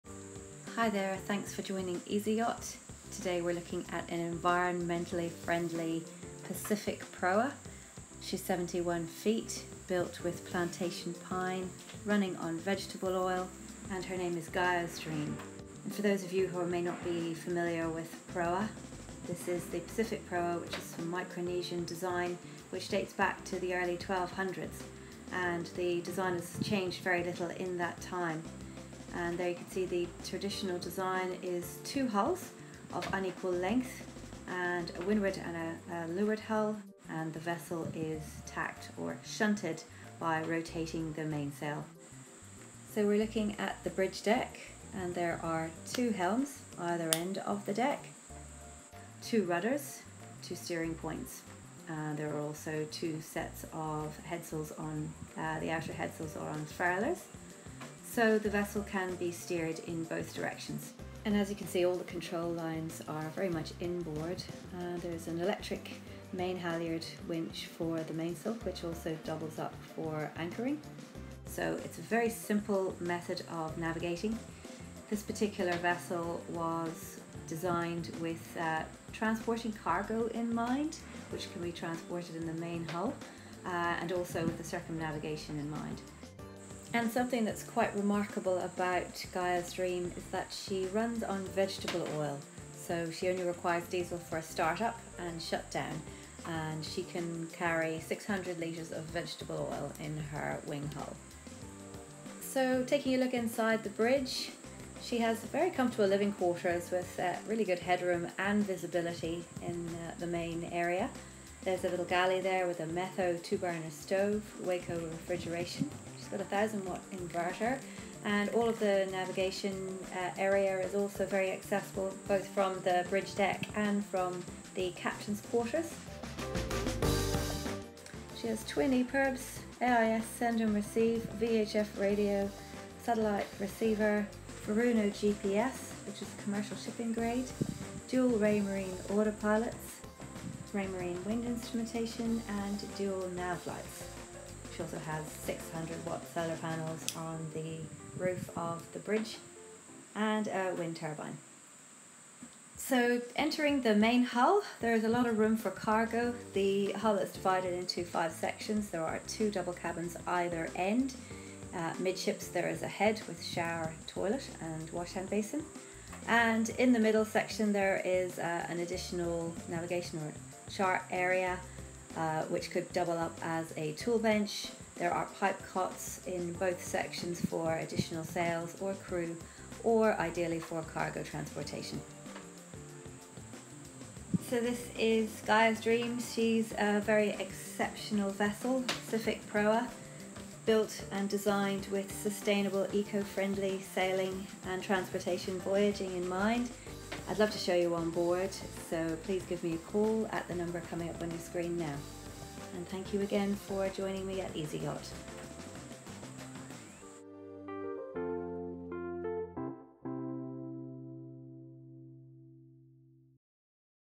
PACIFIC PROA 71 ‘Gaia’s Dream’ virtual tour with EZIYACHT
It's 2020 and time to act on our fossil fuel consumption. If you're keen to minimise your environmental impact seriously, then look to traditional and alternative methods of travel and navigation.

The Pacific Proa was first launched in the 1200's. Usually consisting of two parallel hulls of unequal length, the smaller of the two is kept to windward while tacking involves 'shunting' the main which rotates and the dual direction hull is reversed. Despite the advances in boat building throughout the decades, the Proa's simple construction and sailing technique has little changed, requiring only the wind and a base of water to move at considerable speed.

Gaiasdream* is a Pacific Proa which follows the same principals and design as the traditional proa using sustainable materials for propulsion where we see vegetable oil replacing diesel. Built by esteemed yacht designer and navigator Yvette Wijnen, she is the largest known Proa of her kind in the world.

Designed for transporting cargo and with a circumnavigation in mind Gaisdream was built from Plantation pine using the West system.
*Vessel Sold